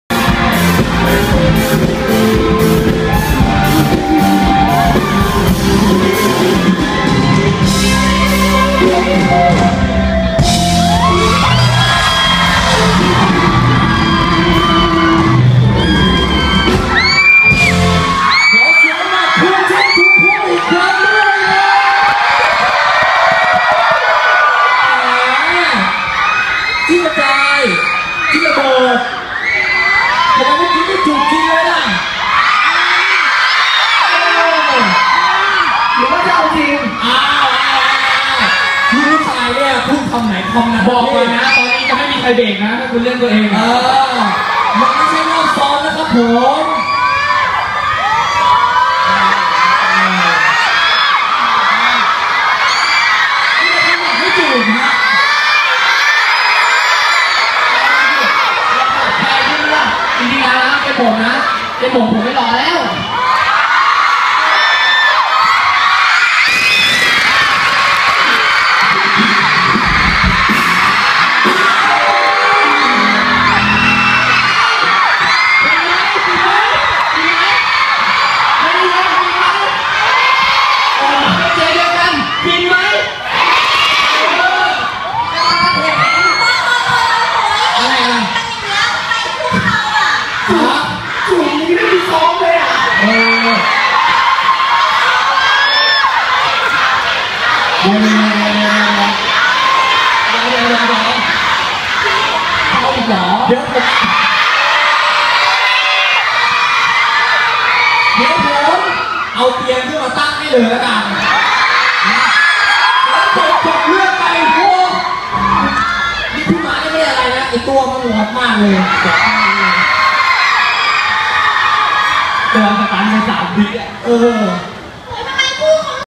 [Vietsub + Engsub] Tousedthawut suddenly kisses Marchutavuth at Hormones Night Day
Video created by gettygate
Vietsub by Me. Please click CC for English subtitle